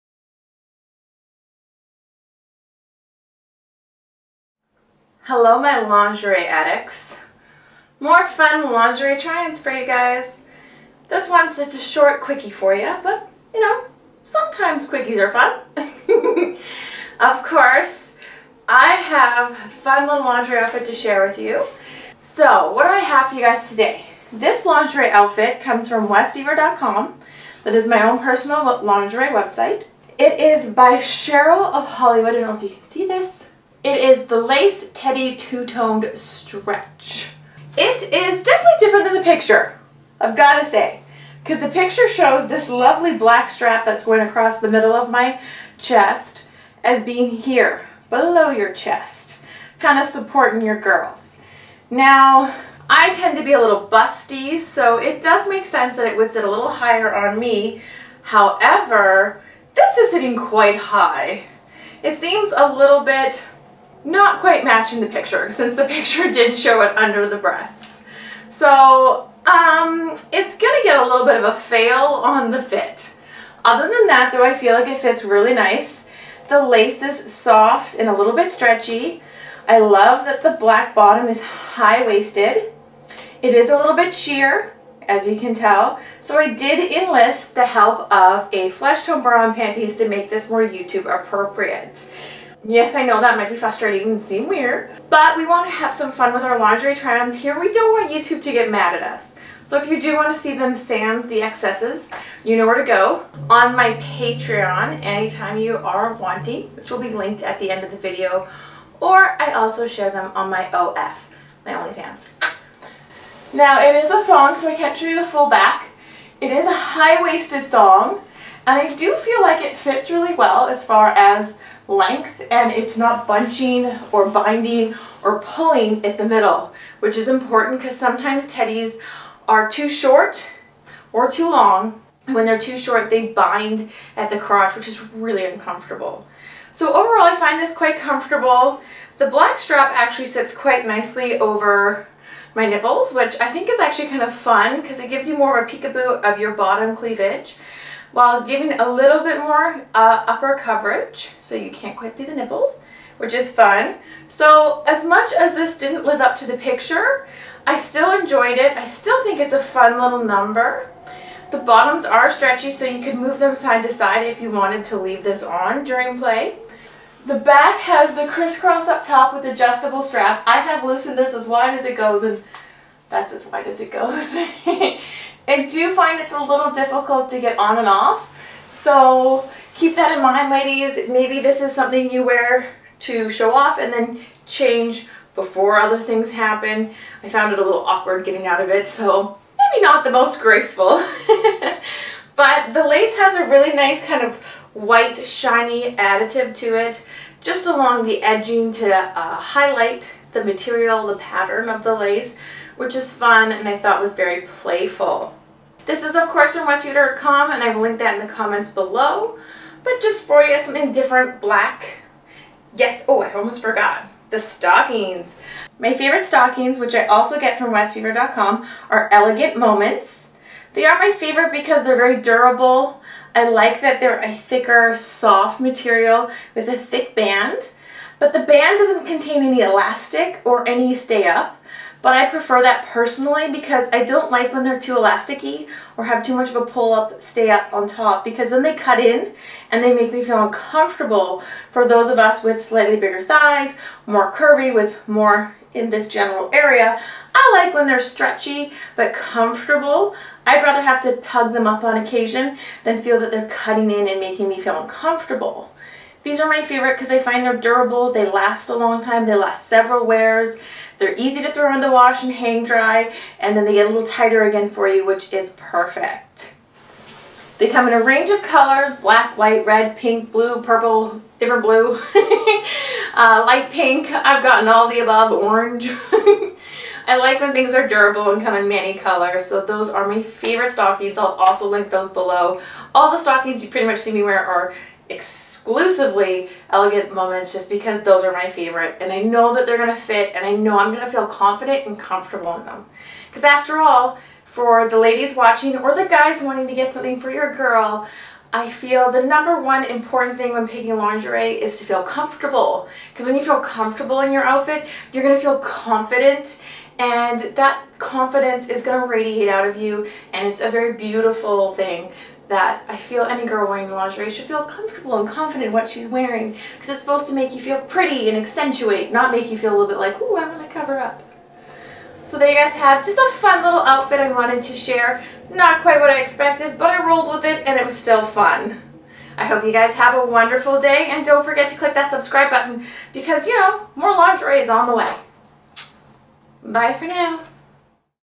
West Fever Summer Lingerie Must have
New lingerie lowdown, must have summer lingerie, curvy girl outfits for mid-life women like myself. This westfever review is for summer must have lingerie that you don't want to miss.

Estalk me I dare you ;)